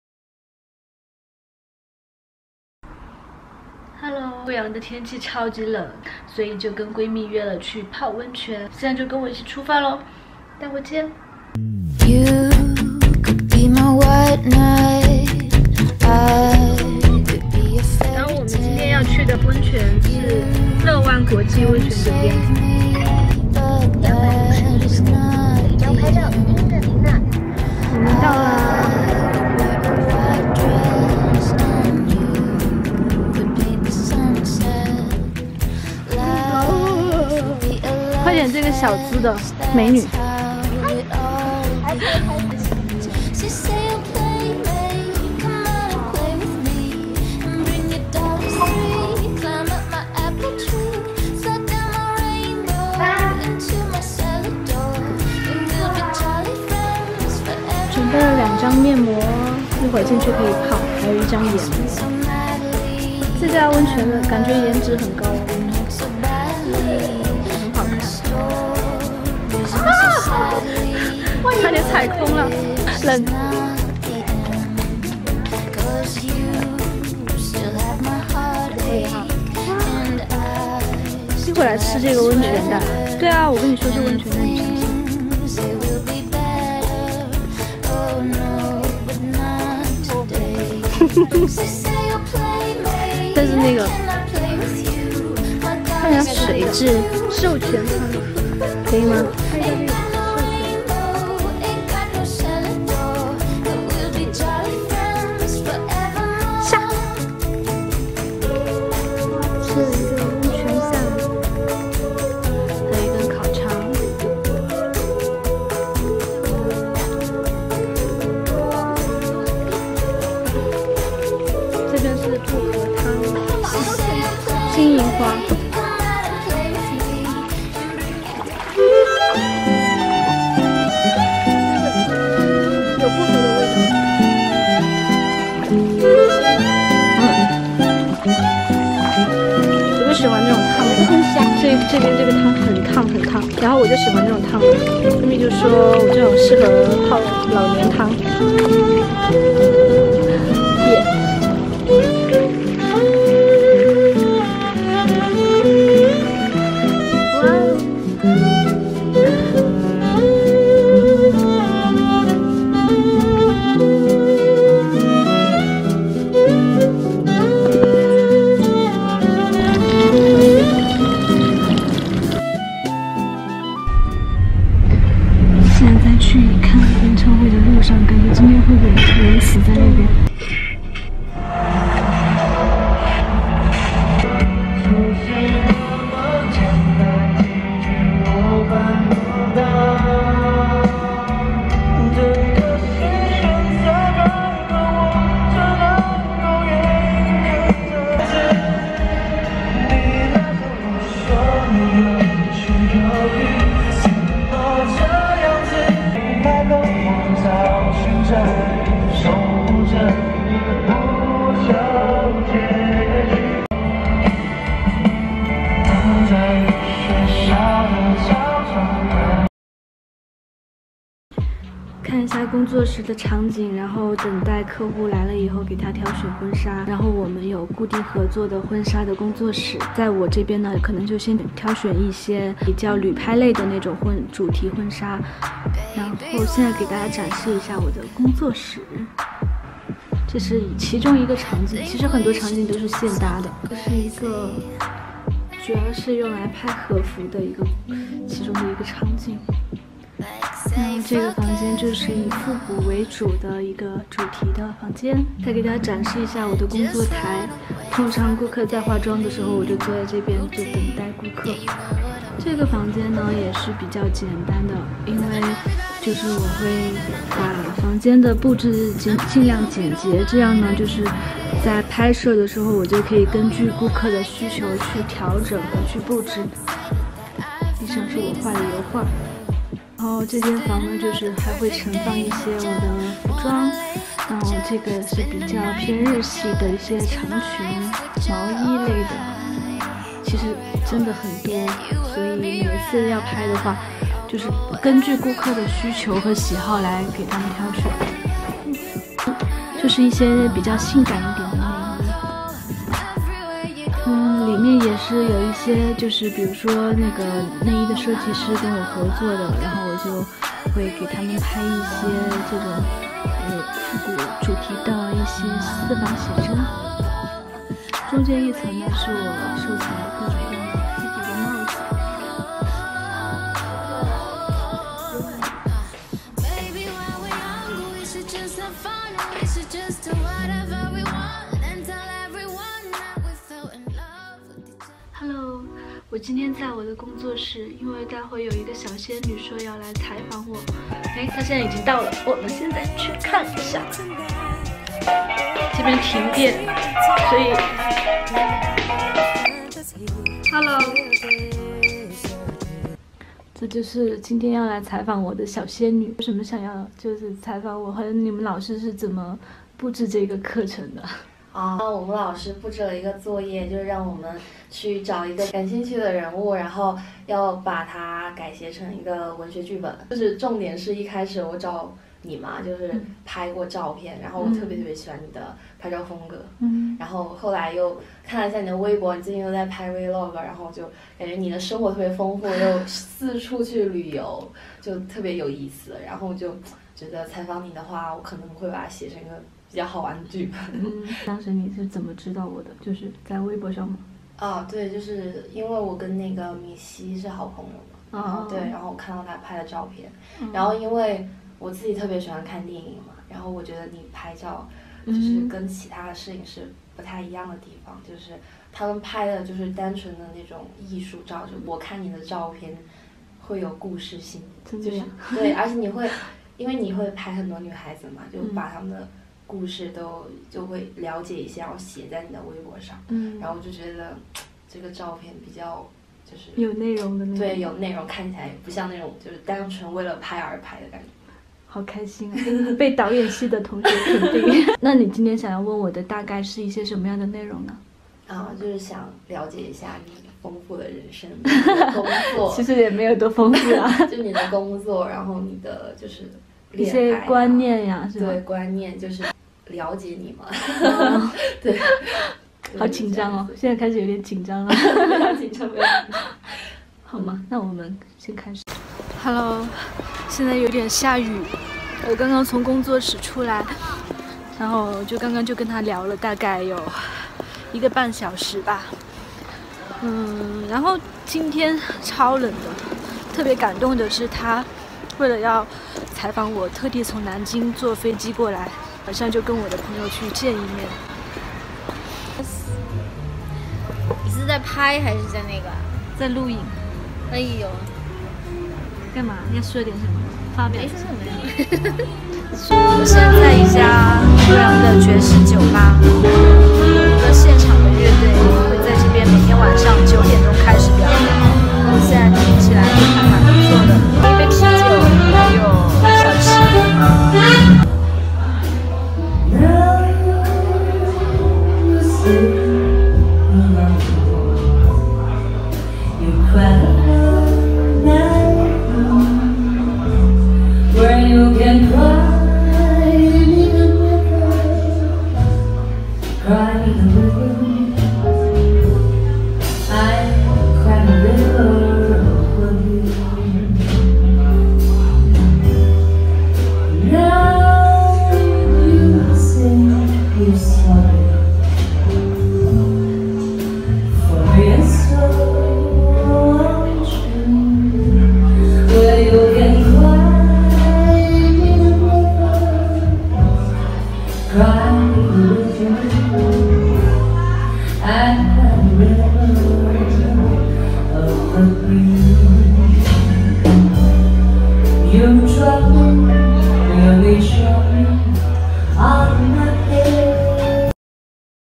宝蓝Vlog｜我的日常 去泡温泉和泡吧你选择哪一个？我选择都去哈哈哈～
我的日常。
泡温泉、陪客户看服装、和闺蜜去泡吧。